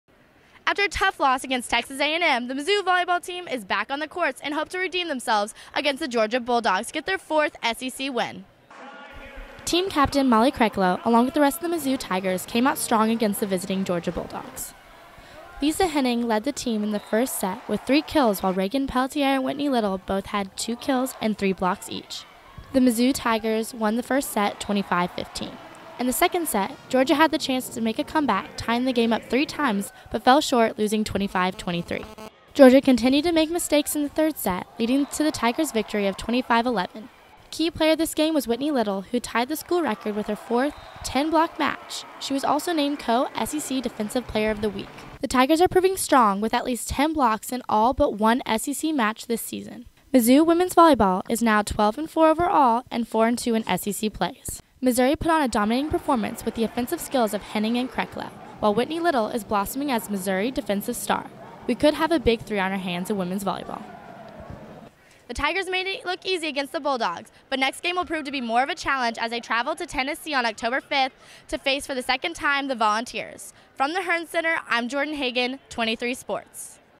Volleyball vs. Georgia 10-1-12
Jordan Hagan brings you all of the highlights from Missouri women's volleyball's latest match against the Georgia Bulldogs.